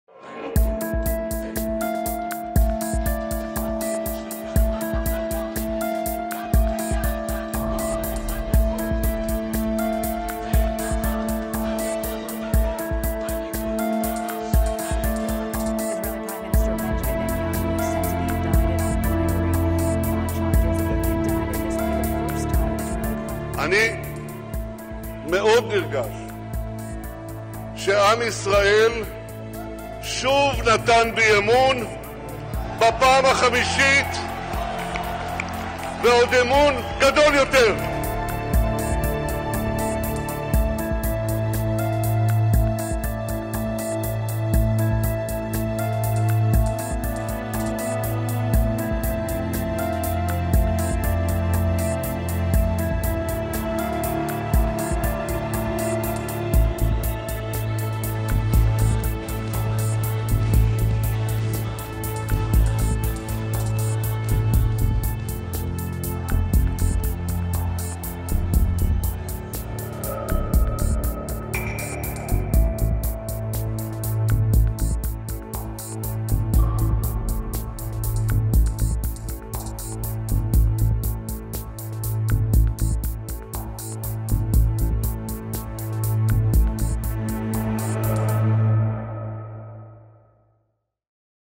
Israel’s Netanyahu secures election victory
Prime Minister Benjamin Netanyahu has won the Israeli national election, securing a record fifth term in office despite running neck in neck with his challenger Benny Gantz, the country’s three main television channels said on Wednesday.